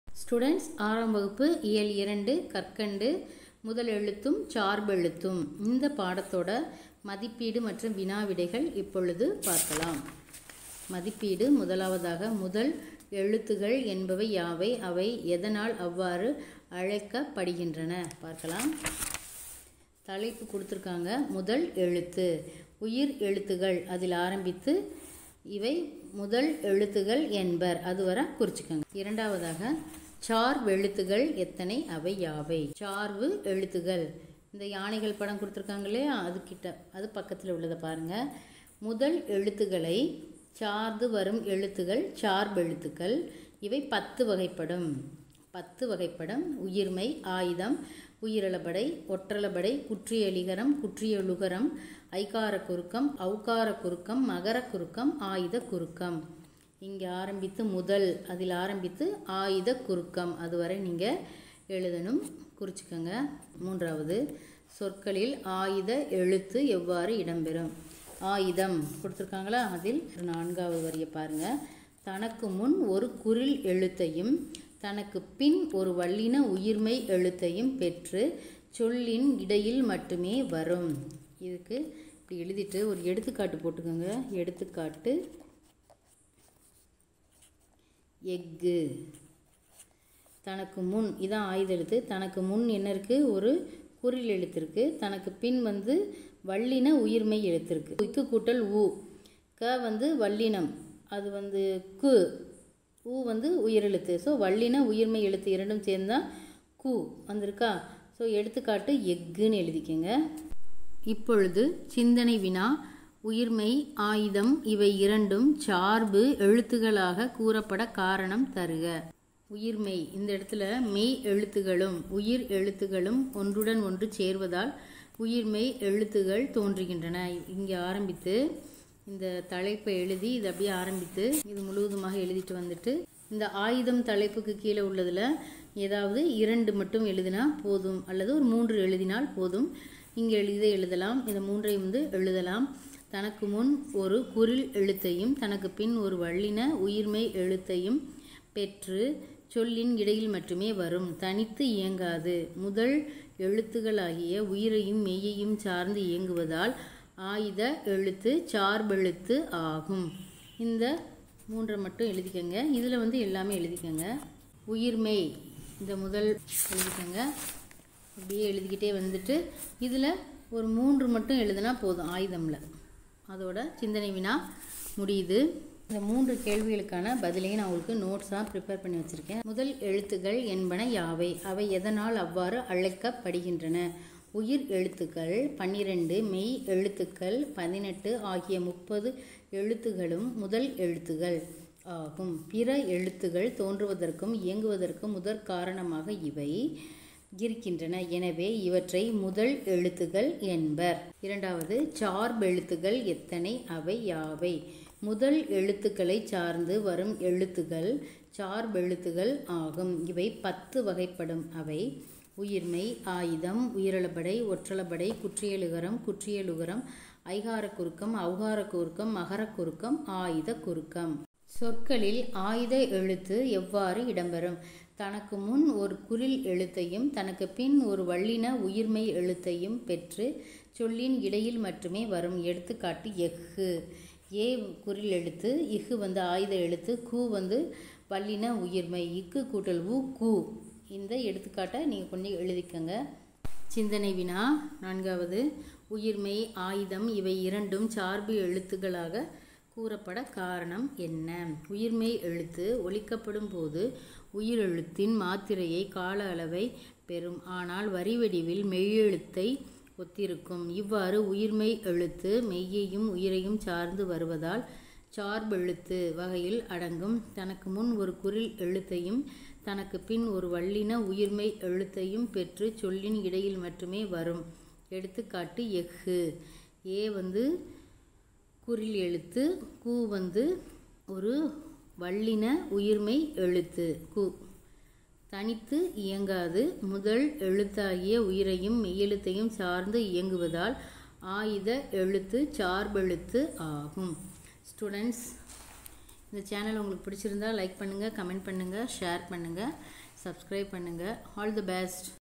6 தமிழ்இயல் 2பருவம் 1 முதலெழுத்தும் சார்பெழுத்தும் மதிப்பீடுவினா விடைகள் சமச்சீர் new syllab2020
வகுப்பு 6 தமிழ் இயல் 2 பருவம் 1 முதலெழுத்தும் சார்பெழுத்தும் மதிப்பீடு (வினா விடைகள்) சமச்சீர் tn new syllabus 2020.. kindly subscribe comment like and share... Jasmine edu corner.. thanks for watching my channel 💐💐💐💐